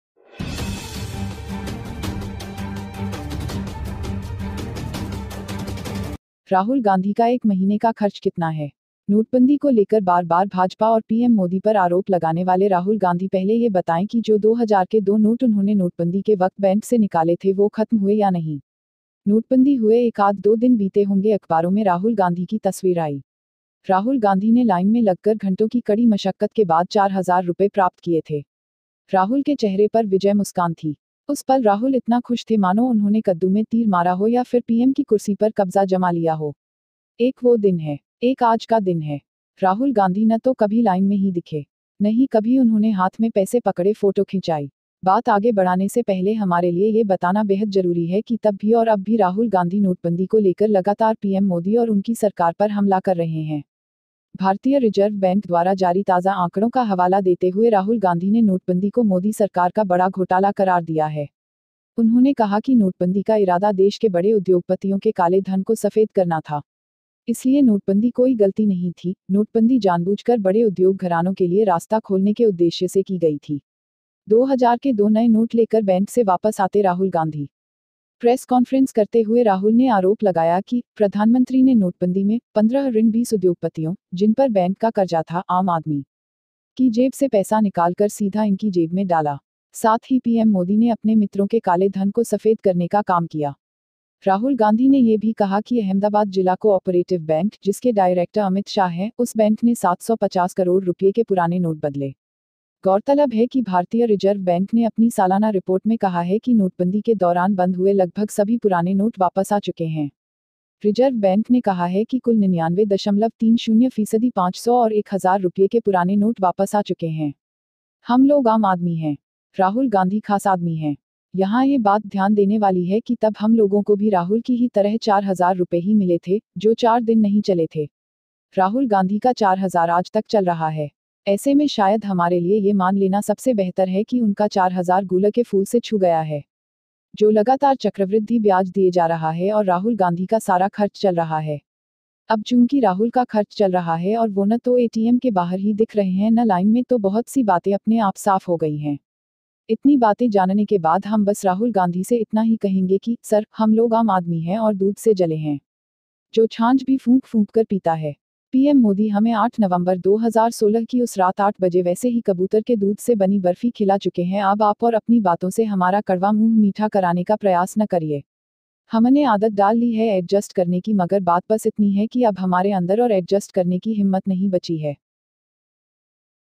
Rahul Gandhi का एक महीने का खर्च कितना है?
How much does Rahul Gandhi spend for one month?

    Entry News

Rahul Gandhi, who repeatedly blamed the BJP and PM Modi for the note-book, should first tell that the 2 thousand 2 notes he had taken out of the bank at the time of the note-offs were over or not?

In the newspapers, Rahul Gandhi's picture came in the newspapers, in the last two or two days, there was a ban on tabla. After spending a lot of money in the line, Rahul Gandhi had received 4000 rupees after the hardships. The victory over Rahul's face was a smile. At that moment, Rahul was so happy as if he had hit an arrow in a pumpkin or had taken possession of the PM's chair.

That's one day. Today is a day. Rahul Gandhi never appeared in line. Nor did he ever grab the money holding money in his hand. Before moving forward, it is very necessary for us to tell that even then and now even Rahul Gandhi is constantly attacking PM Modi and his government about the ban on the ban.

Referring to the latest data released by the Reserve Bank of India, Rahul Gandhi termed the ban on a big scam of the Modi government. He said that the intention of the ban was to whiten the black money of the big industrialists of the country. Therefore there was no mistake in the ban on bondage, the ban was deliberately aimed at opening the path for big industry houses.

Rahul Gandhi coming back from the bank with two new notes of two thousand

While making a press conference, Rahul alleged that, in the note-taking of the Prime Minister, 15-20 industrialists, who had bank loan, took out money from the common man's pocket and put them in their pocket directly. In addition, PM Modi worked to whiten the money of his friends. Rahul Gandhi also said that the Ahmedabad district co-operative bank, whose director Amit Shah, changed the old notes of Rs 750 crores. Significantly, the Reserve Bank of India has said in its annual report that almost all old notes that were closed during the note-off have returned. The Reserve Bank has said that 99.30 percent of the total notes of 500 and 1000 rupees have come back.

We are common people. Rahul Gandhi is a special man. It is worth mentioning here that we had received only 4000 rupees like Rahul Gandhi, who did not go for four days. Rahul Gandhi's 4000 is going on till today. In such a case, it is best for us to believe that their 4000 touches the flower of Gular. The continuous compound interest is being given and Rahul Gandhi is spending all the time. Now that Rahul's expenditure is going on and he is not seen out of the ATM too many things have been cleared by himself in the line.

After knowing such things, we will just say to Rahul Gandhi, "Sir, we are ordinary people and are burnt with milk, which also whispers and drinks." PM Modi has given us Barfi made of pigeon milk at 8 o'clock that night, 8th November 2016. Now do not try to sweat our bitter mouth with you and your talk. We have habited to adjust but the only thing to do is that we no longer have the courage to adjust inside us.